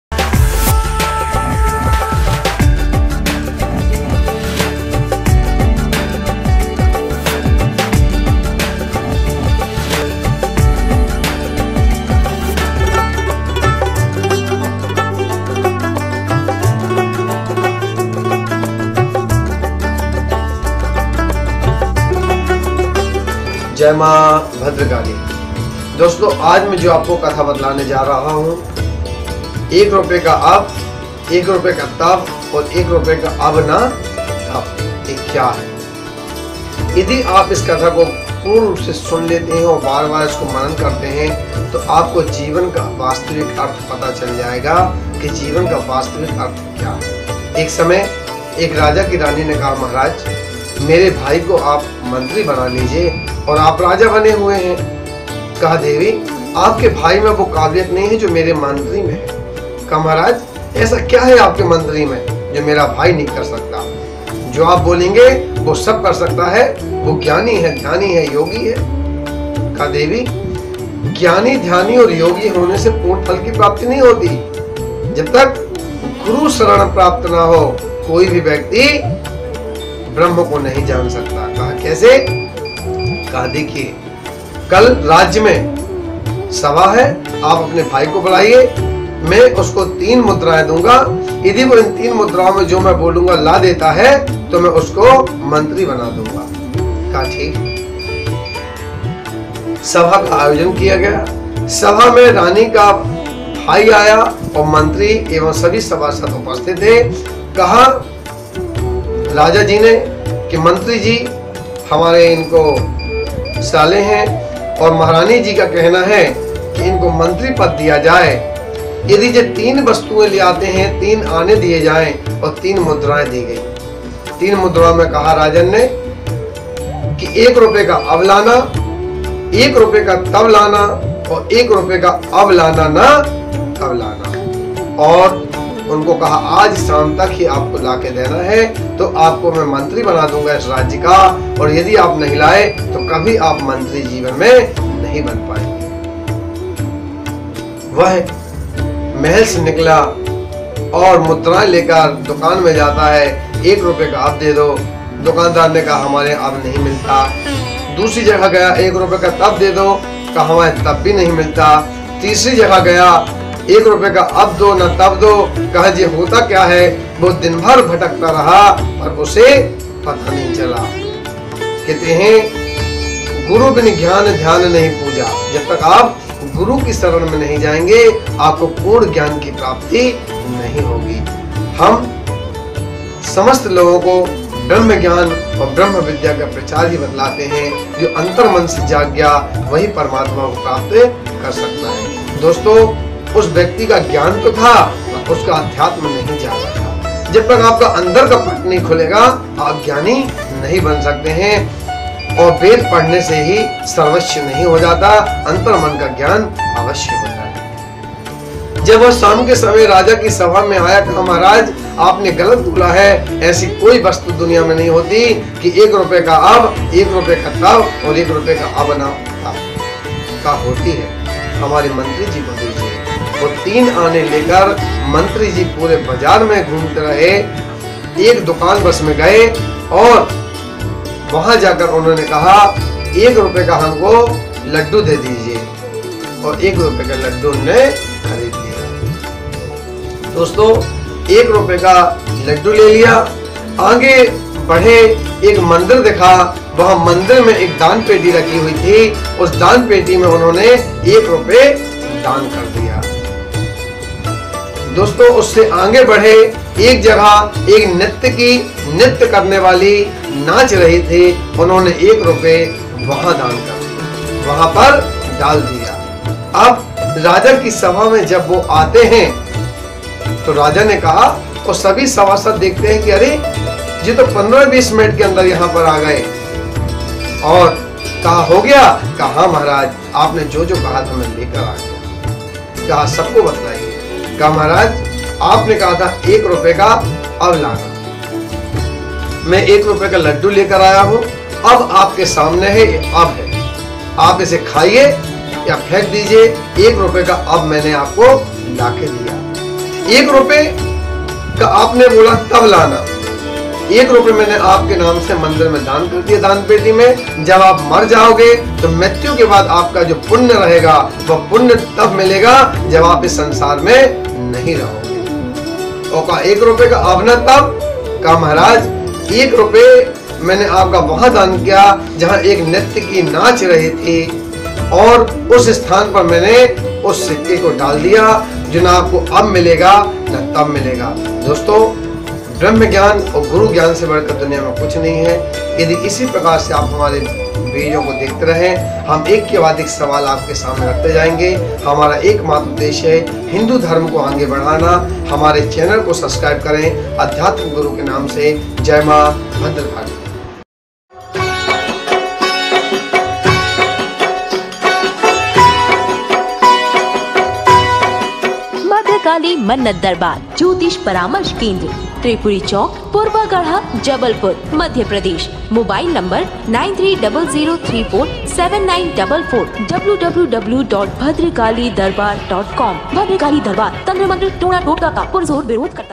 राजा ने रानी के भाई को मंत्री बनाने रखी अनोखी शर्त, जानें पूरी कहानी
जीवन का गूढ़ रहस्य है इस कहानी में